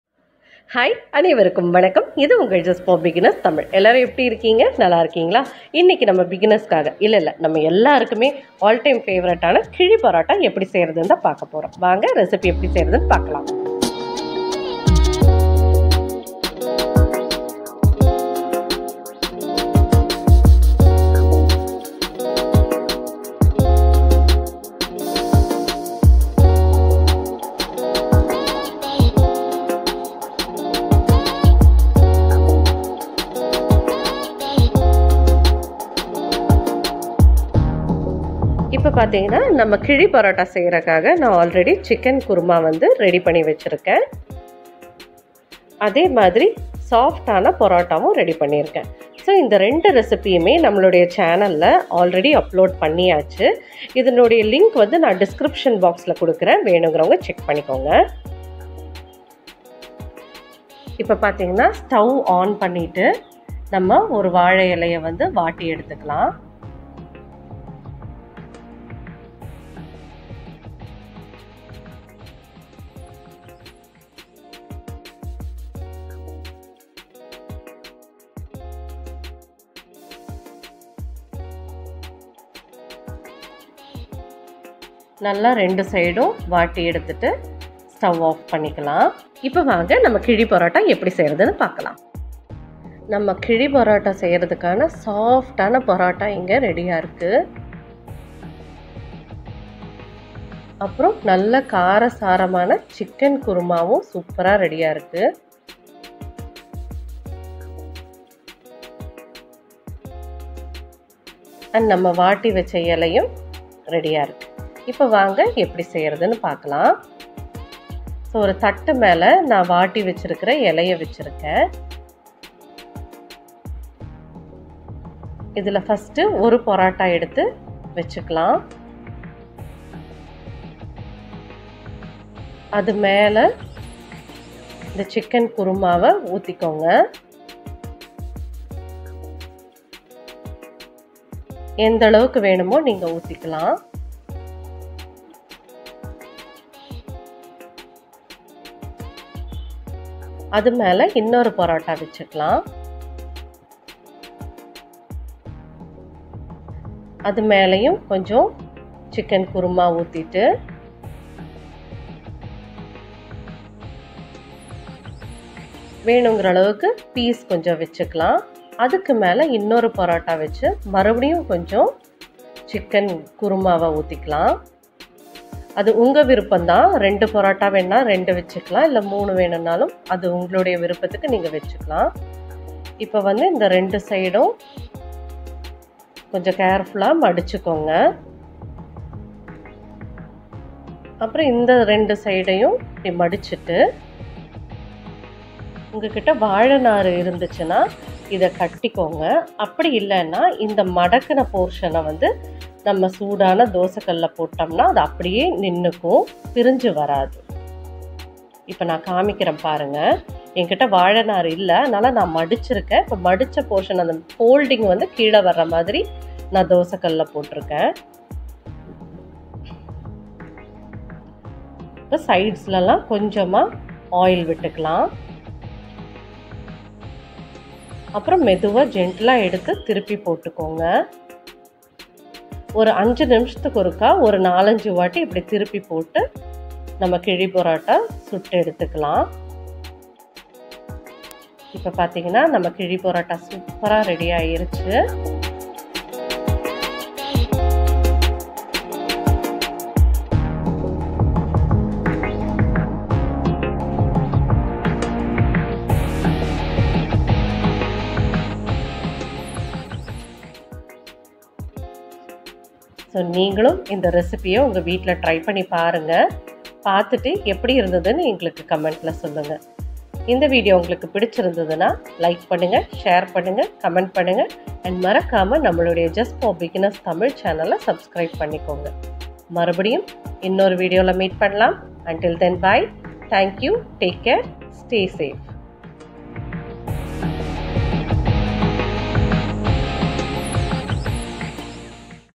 Vazha elai / Kizhi parotta recipe || Potlam parotta recipe in Tamil || Just for beginners Tamil
Hope all you are doing well..

Today I have come with a very delicious , easy recipe "Vazha elai poratta / Kizhi parotta " recipe. ..
This Vazha elai poratta / Kizhi parotta recipe is also called "Potlam parotta "
This "Vazha elai poratta / Kizhi parotta" recipe is an excellent and complete weekend recipe...
This "Vazha elai poratta / Kizhi parotta" recipe goes well with Chicken kuruma , Mutton kuruma , Egg gravy , onion raitha,etc...

Tip :
*You may also add Mutton gravy, egg gravy, etc.. Any gravy (Veg /non veg) is also recommend instead of chicken gravy..

Check out the videos of -

Do try this "Vazha elai poratta / Kizhi parotta " recipe at your home and share your feedback, opinion and comments on the " Vazha elai poratta / Kizhi parotta l recipe in the comment section..

Meet you soon in an another interesting video until then "Bye" ,"Thank you" ,"Take care"
- Just for beginners Tamil